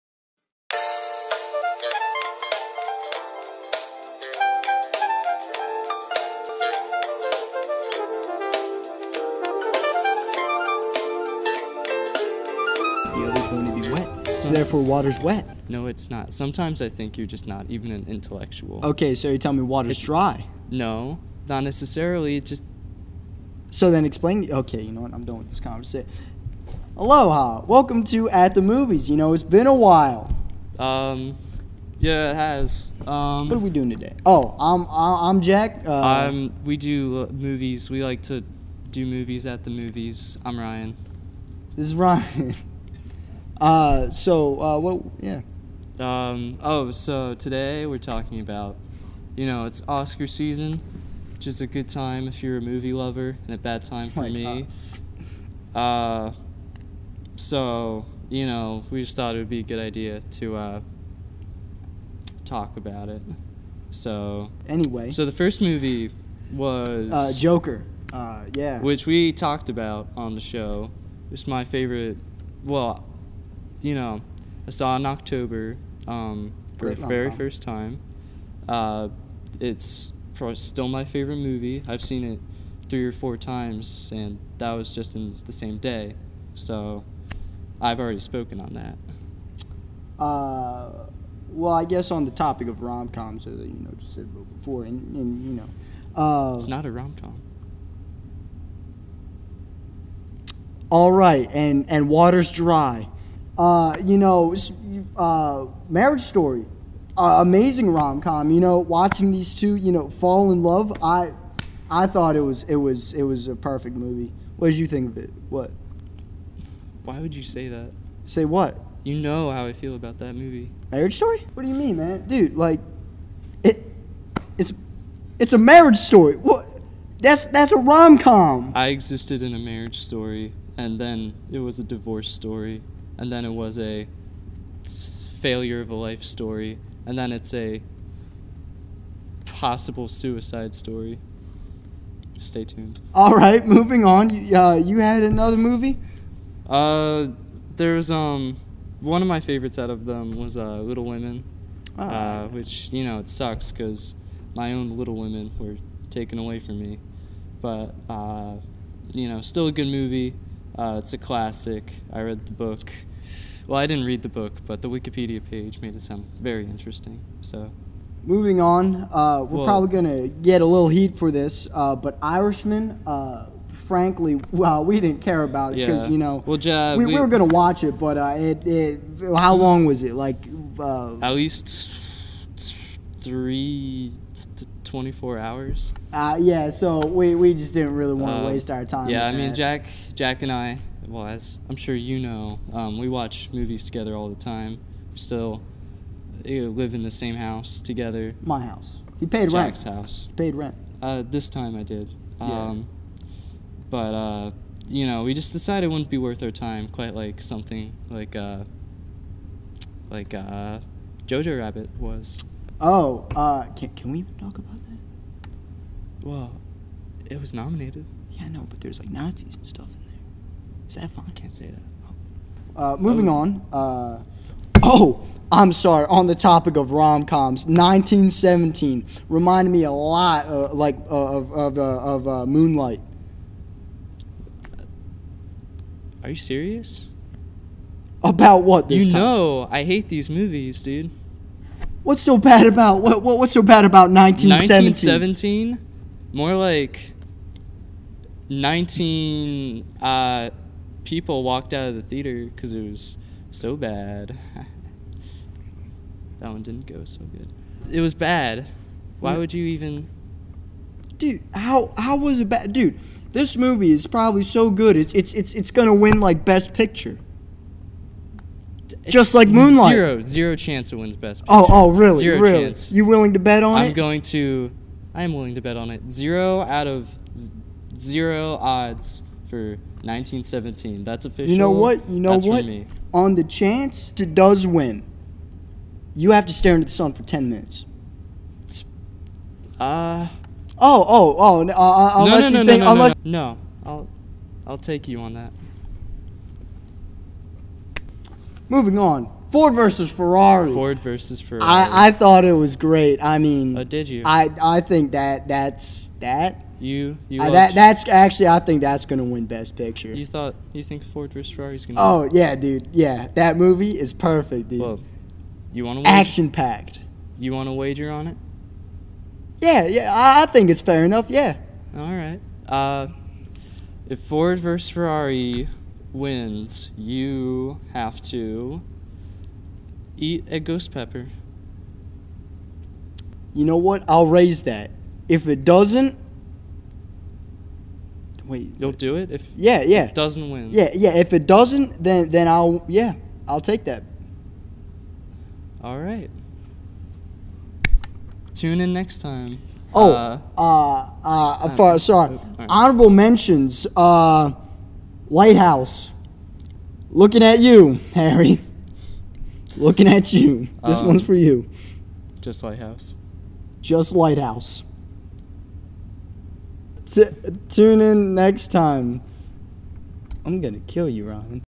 At The Movies: Oscar Nominations
Jack and Ryan provide their collective knowledge to the world of prestige film awards. They comment on each of the nominees except for that foreign trash one or the commie tarantino one.